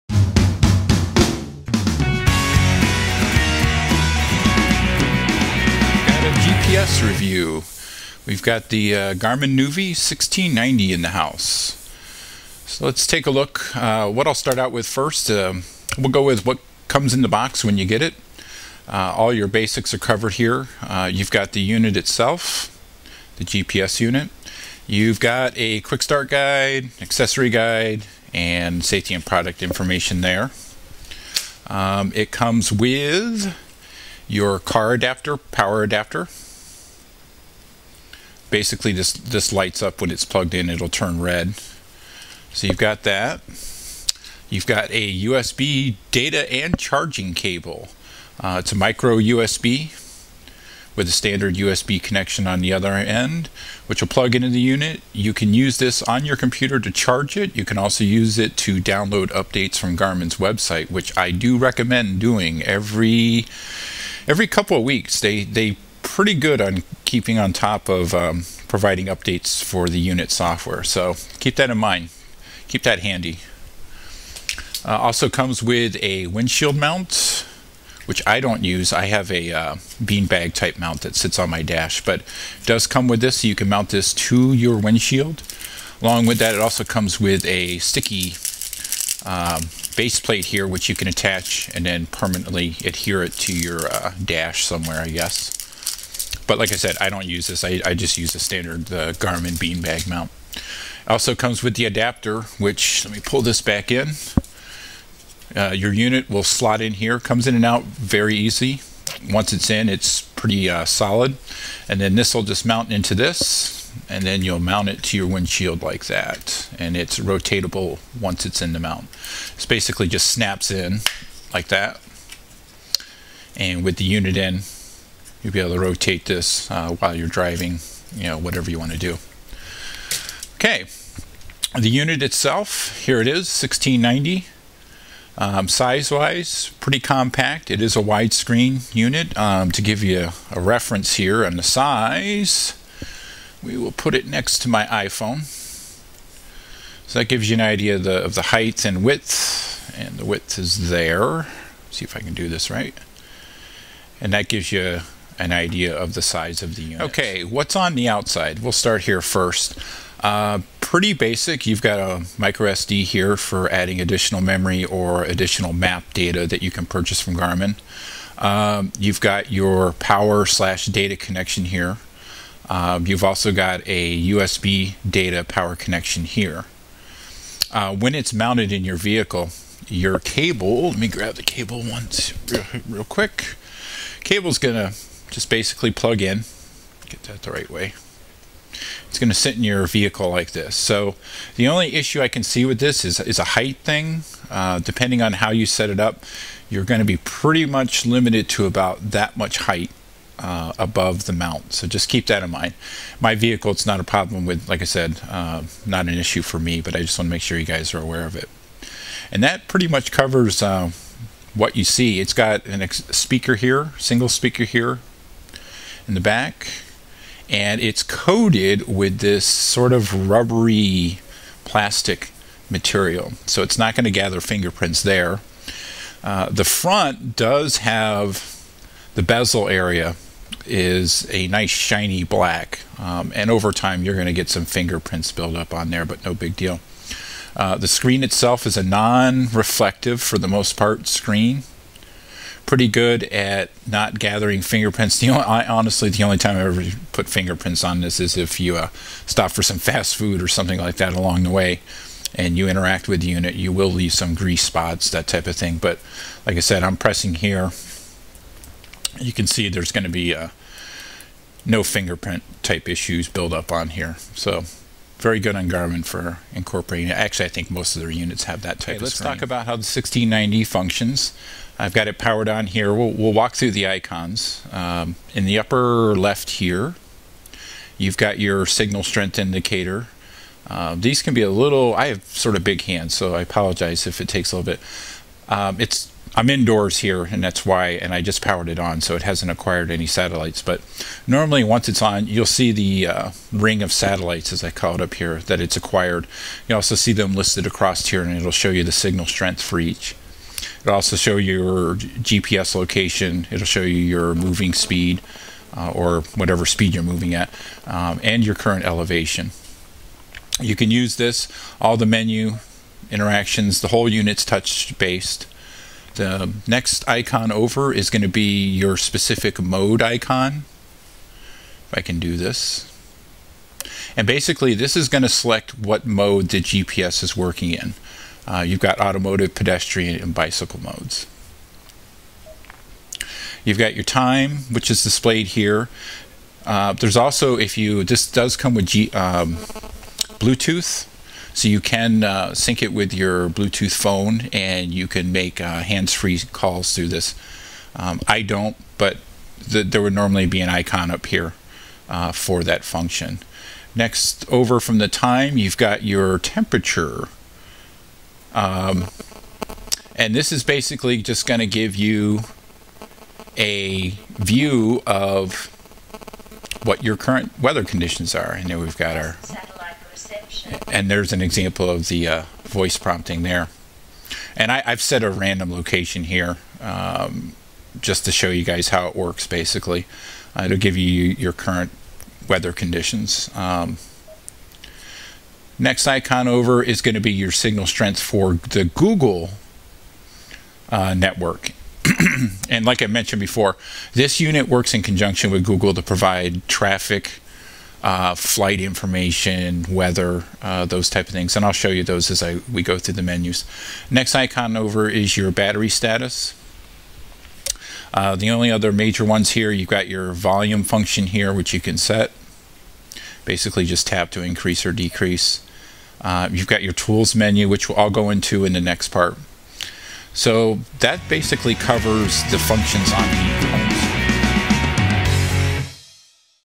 Garmin Nuvi 1690 Overview & Review - Part 1
Garmin Nuvi 1690 overview and review. Part 1 covers the unit itself, home screen icons and basic operation.